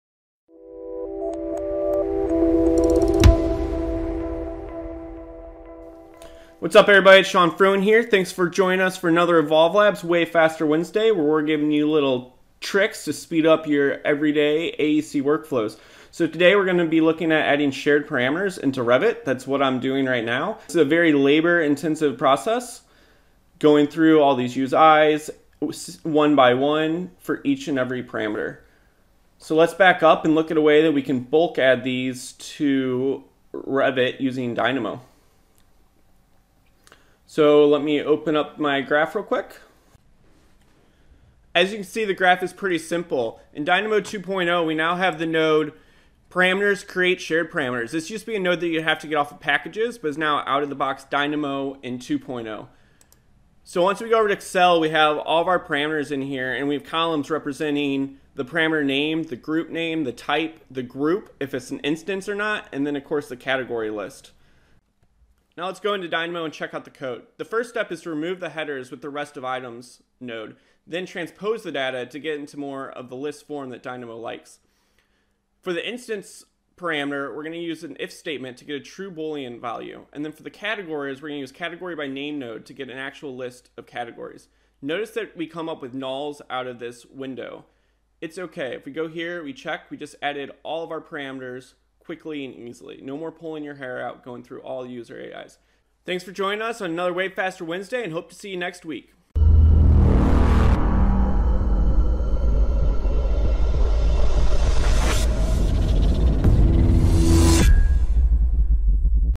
Dynamo Shared Parameters and Project Parameters - Way Faster Wednesday
It's not a secret - Adding Shared Parameters and Project Parameters is not a fun task. Using Dynamo, you can bulk add shared parameters and project parameters from an Excel file saving tons of time! Sean Fruin walks you through it here!

Want more free training videos?